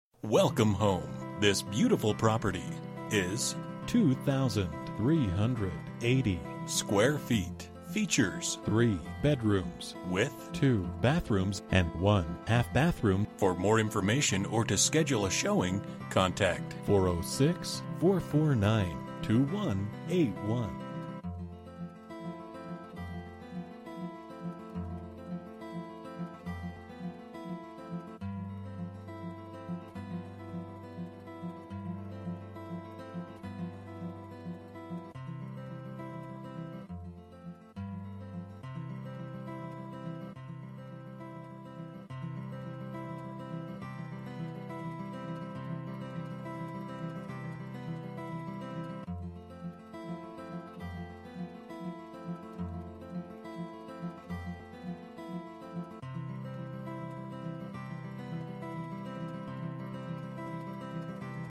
Residential for sale - 2824 Stacia Avenue, Helena, MT 59601
Excellent location in Craftsman Village. Right next to the paved walking path that leads to the massive community park- 1 block away!! This home has EVERY available upgrade: granite, hardwood, A/C, the works!! This home is priced to sell TODAY! Call ASAP for your showing!

Bedrooms: 3
Bathrooms: 2 full and 1 half Bathrooms
Square Feet: 2380
Price: $254,900